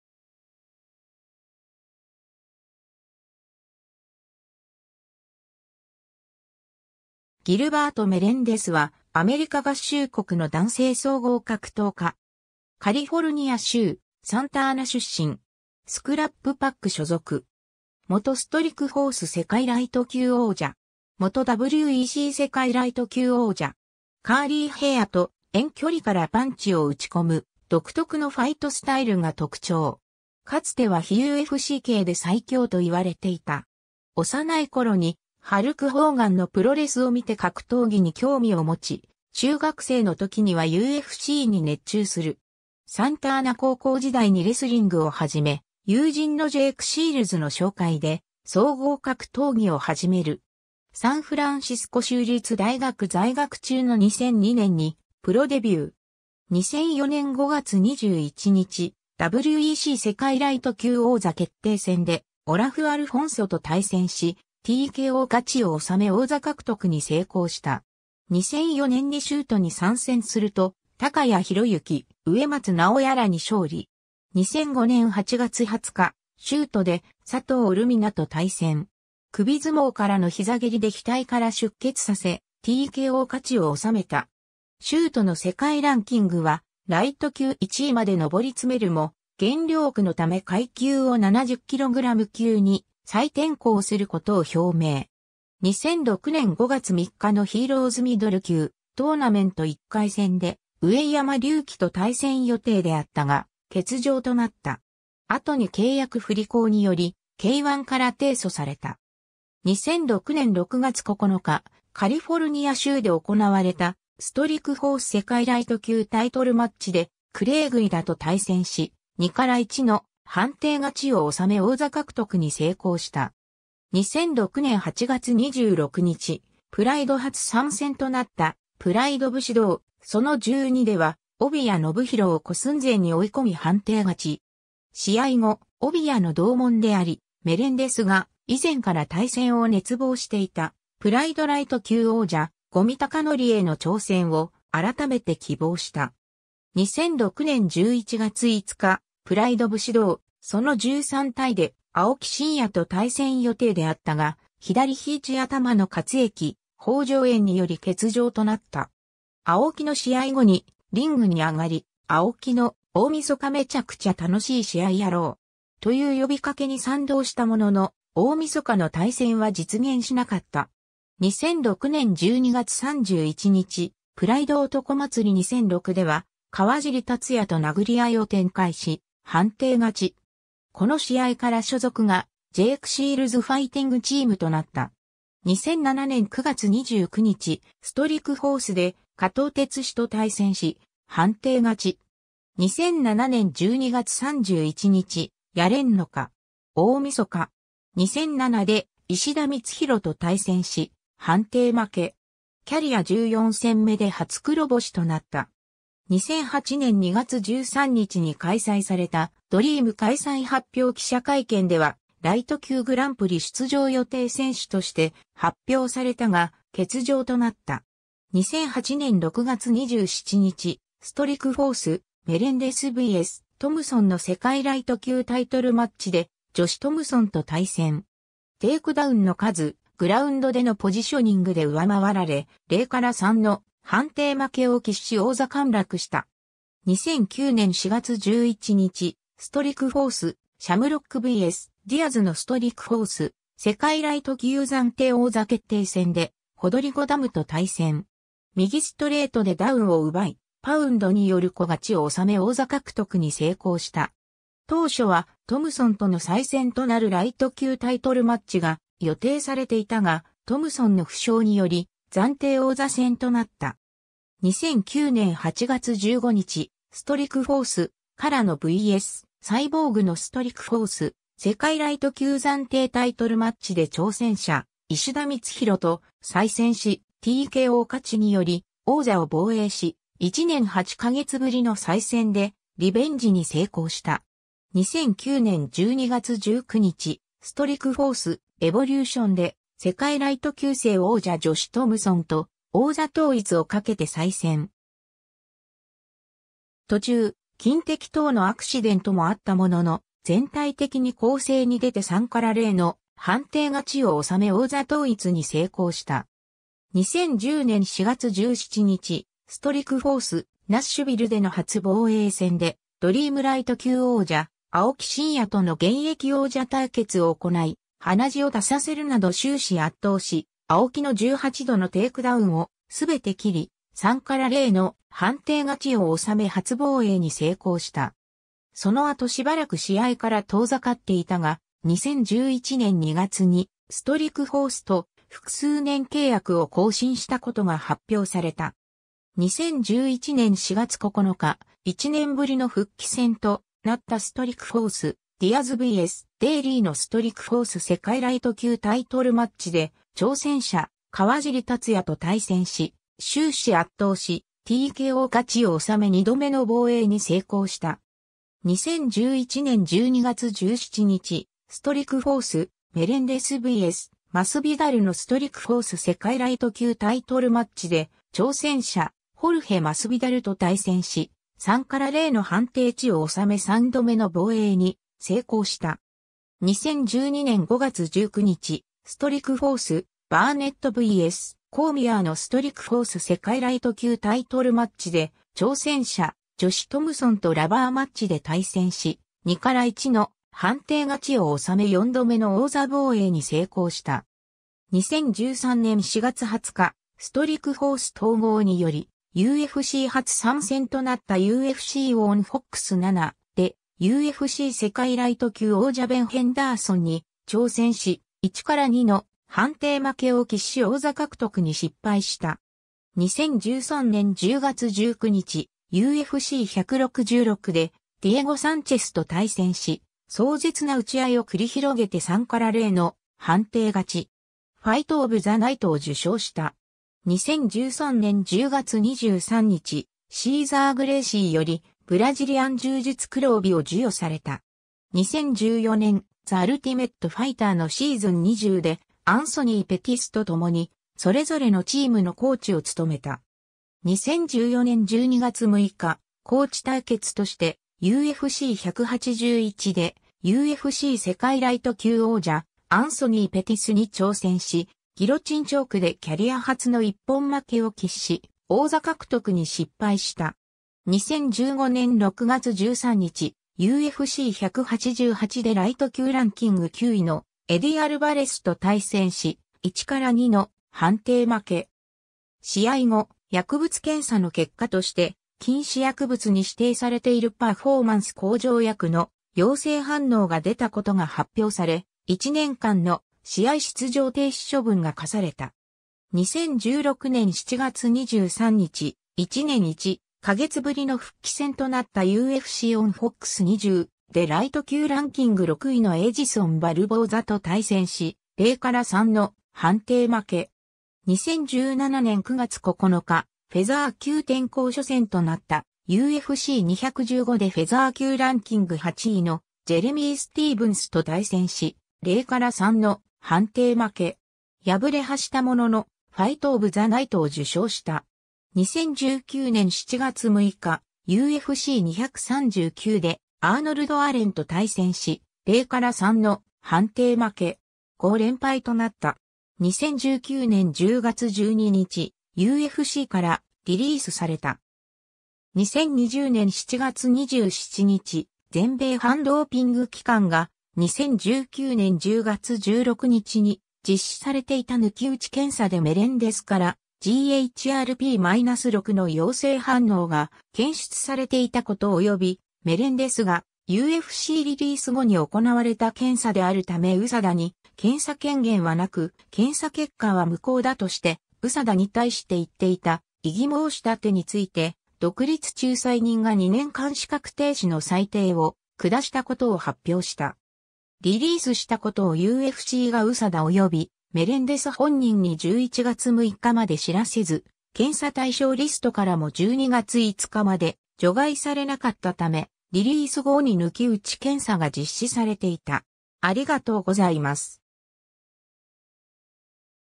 ギルバート・メレンデス
ギルバート・メレンデス（Gilbert Melendez、1982年4月12日 - ）は、アメリカ合衆国の男性総合格闘家。
カリフォルニア州サンタアナ出身。
スクラップ・パック所属。
元Strikeforce世界ライト級王者。
元WEC世界ライト級王者。
 カーリーヘアと遠距離からパンチを打ち込む独特のファイトスタイルが特徴。
かつては非UFC系で最強と言われていた。
 幼い頃にハルク・ホーガンのプロレスを見て格闘技に興味を持ち、中学生の時にはUFCに熱中する。
サンタアナ高校時代にレスリングを始め、友人のジェイク・シールズの紹介で総合格闘技を始める。
サンフランシスコ州立大学在学中の2002年にプロデビュー。
 2004年5月21日、WEC世界ライト級王座決定戦でオラフ・アルフォンソと対戦し、TKO勝ちを収め王座獲得に成功した。
 2004年に修斗に参戦すると、高谷裕之、植松直哉らに勝利。
 2005年8月20日、修斗で佐藤ルミナと対戦。
首相撲からの膝蹴りで額から出血させ、TKO勝ちを収めた。
 修斗の世界ランキングはライト級1位まで上り詰めるも、減量苦のため階級を70kg級に再転向することを表明。
 2006年5月3日のHERO'Sミドル級（-70kg）トーナメント1回戦で上山龍紀と対戦予定であったが、欠場となった。
後に契約不履行によりK-1から提訴された。
 2006年6月9日、カリフォルニア州で行われたStrikeforce世界ライト級タイトルマッチでクレイ・グイダと対戦し、2-1の判定勝ちを収め王座獲得に成功した。
 2006年8月26日、PRIDE初参戦となった『PRIDE 武士道 -其の十二-』では帯谷信弘をKO寸前に追い込み判定勝ち。
試合後、帯谷の同門であり、メレンデスが以前から対戦を熱望していたPRIDEライト級王者・五味隆典への挑戦を改めて希望した。
 2006年11月5日、PRIDE 武士道 -其の十三-で青木真也と対戦予定であったが、左肘頭の滑液包襄炎により欠場となった。
青木の試合後にリングに上がり、青木の「大晦日メチャクチャ楽しい試合やろう。」という呼びかけに賛同したものの、大晦日の対戦は実現しなかった。
 2006年12月31日、PRIDE 男祭り 2006では川尻達也と殴り合いを展開し、判定勝ち。
この試合から所属がジェイク・シールズ・ファイティング・チームとなった。
 2007年9月29日、Strikeforceで加藤鉄史と対戦し、判定勝ち。
 2007年12月31日、やれんのか! 大晦日! 2007で石田光洋と対戦し、判定負け。
キャリア14戦目で初黒星となった。
 2008年2月13日に開催されたDREAM開催発表記者会見では、ライト級グランプリ出場予定選手として発表されたが、欠場となった。
 2008年6月27日、Strikeforce: Melendez vs. Thomsonの世界ライト級タイトルマッチでジョシュ・トムソンと対戦。
テイクダウンの数、グラウンドでのポジショニングで上回られ、0-3の判定負けを喫し王座陥落した。
 2009年4月11日、Strikeforce: Shamrock vs. DiazのStrikeforce世界ライト級暫定王座決定戦でホドリゴ・ダムと対戦。
右ストレートでダウンを奪い、パウンドによるKO勝ちを収め王座獲得に成功した。
当初はトムソンとの再戦となるライト級タイトルマッチが予定されていたが、トムソンの負傷により暫定王座戦となった。
 2009年8月15日、Strikeforce: Carano vs. CyborgのStrikeforce世界ライト級暫定タイトルマッチで挑戦者石田光洋と再戦し、TKO勝ちにより王座を防衛し、1年8か月ぶりの再戦でリベンジに成功した。
 2009年12月19日、Strikeforce: Evolutionで世界ライト級正王者ジョシュ・トムソンと王座統一を賭けて再戦。
途中、金的等のアクシデントもあったものの、全体的に攻勢に出て3-0の判定勝ちを収め王座統一に成功した。
 2010年4月17日、Strikeforce: Nashvilleでの初防衛戦でDREAMライト級王者・青木真也との現役王者...